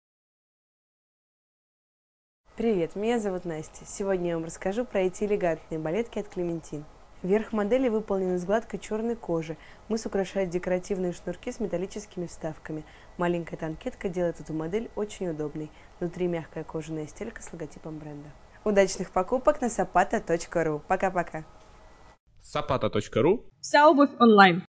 Балетки Clementine арт. DJH43
Видеообзор на балетки Clementine арт. DJH43
цвет - черный, сезон - лето, внешний материал - натуральная кожа, внутренний материал - натуральная кожа.